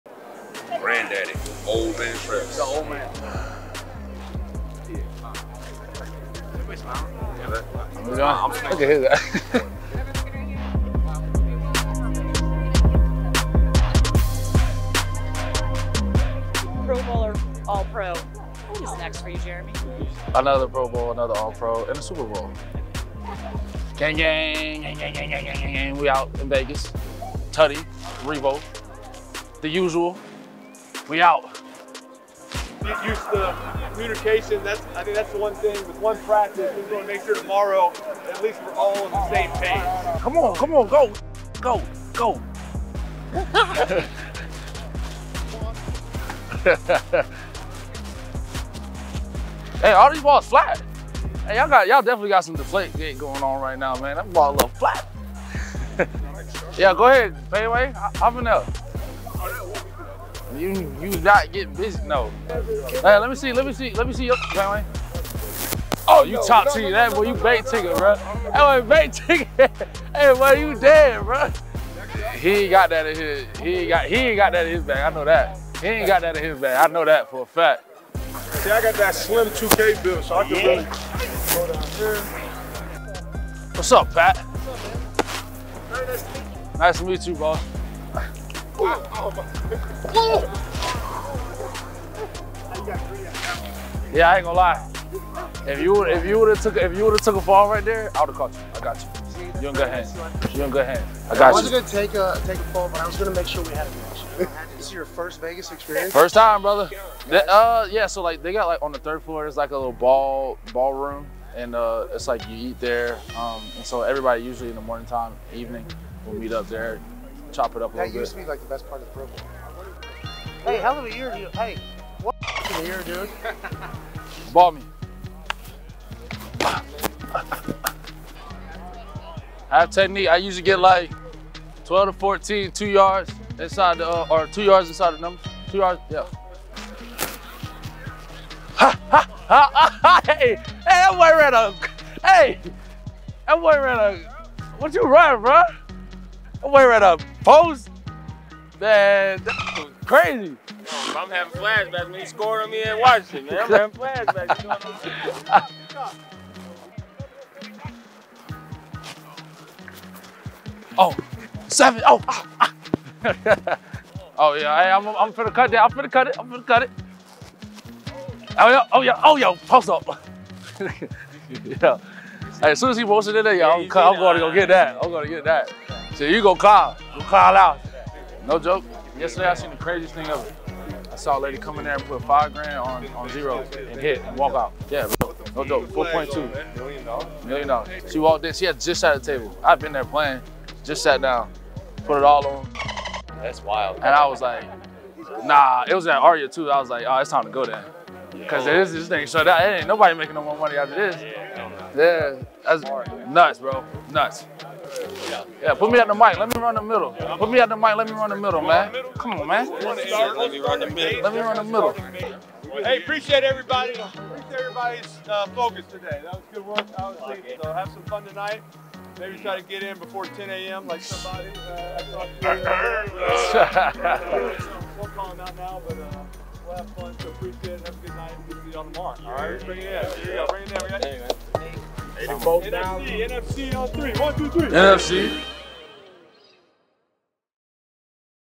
ALL-PRO REAVO | Jeremy Reaves mic'd up for Pro Bowl practice
We had Jeremy Reaves mic'd up at a Pro Bowl Games practice

Presented by Easterns